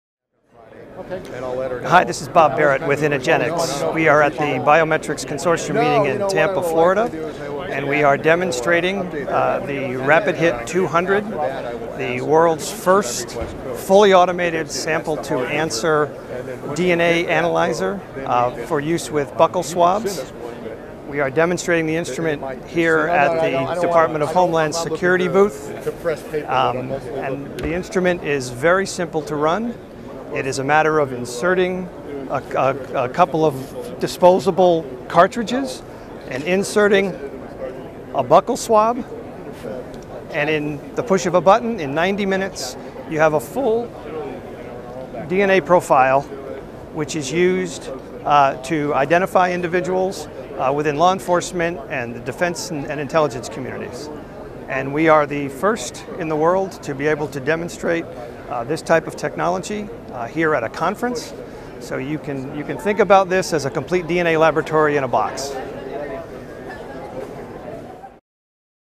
integenX Showcases their RapidHIT 200 Fully Automated DNA Identification System at BCC 2012
integenX showcases their RapidHIT 200 fully automated DNA identification system at the 2012 Biometric Consortium Conference